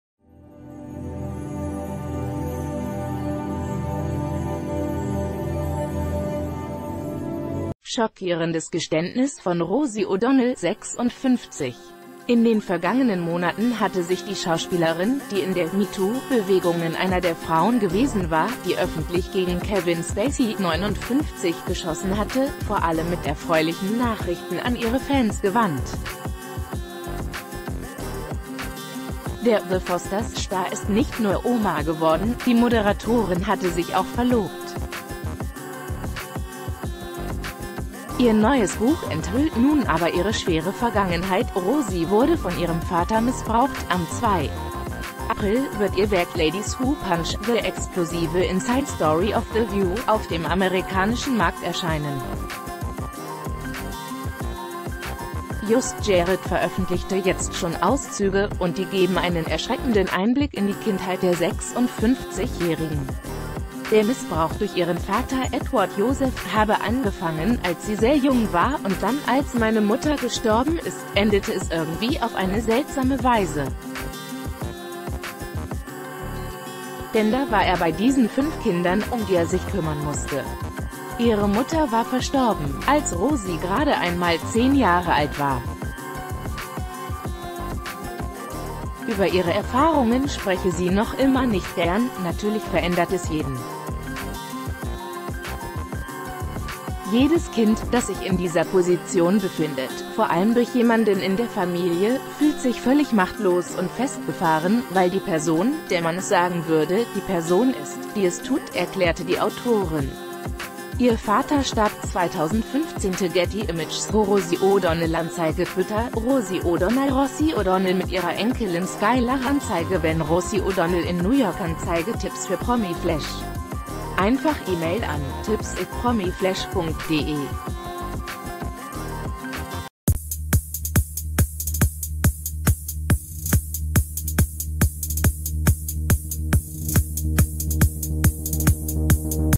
Rosie O'Donnell wurde als Kind von eigenem Vater missbraucht
Schockierendes Geständnis von Rosie O'Donnell (56)! In den vergangenen Monaten hatte sich die Schauspielerin, die in der "MeToo"-Bewegungen einer der Frauen gewesen war, die öffentlich gegen Kevin Spacey (59) geschossen hatte, vor allem mit erfreulichen Nachrichten an ihre Fans gewandt. Der "The Fos...